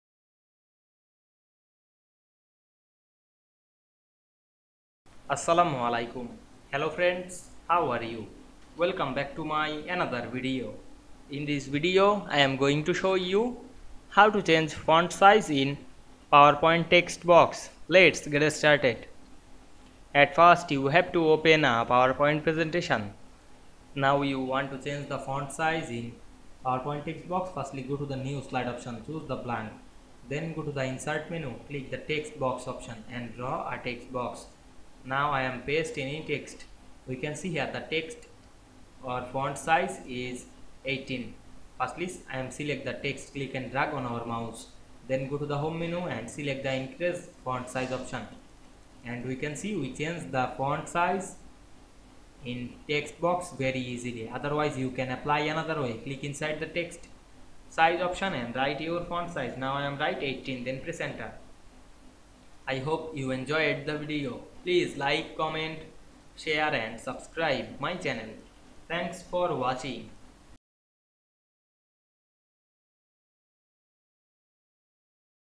How to change font size in PowerPoint text box
Assalamu Walaikum,
In this video I will show you, How to change font size in PowerPoint text box.  Let's get started.